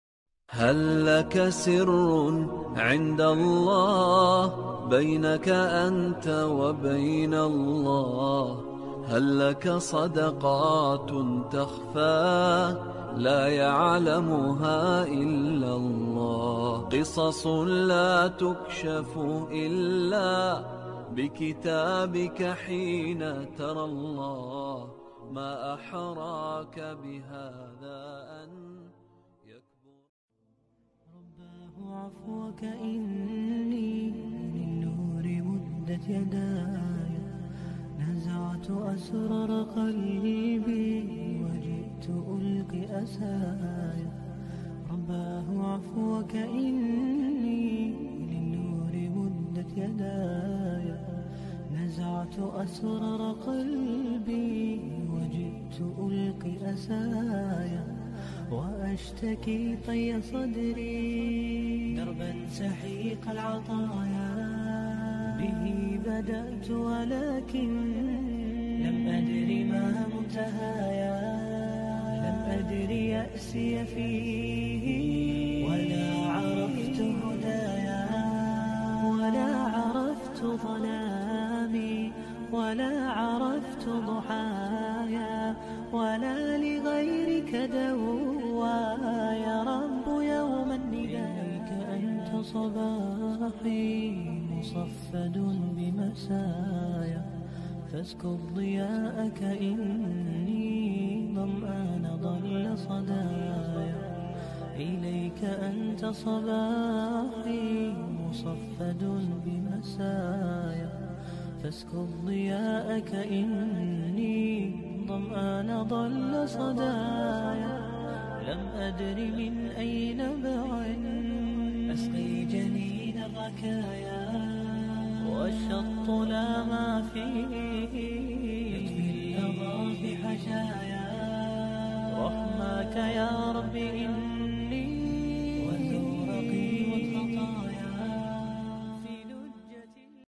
Sajdah of Shukr / Sujud Syukur (Prostration of Gratitude to Allah).
What is Sujud Syukur?
Why do we need to do prostration of gratitude?
How to do Sajdah Shukr?
When to perform this?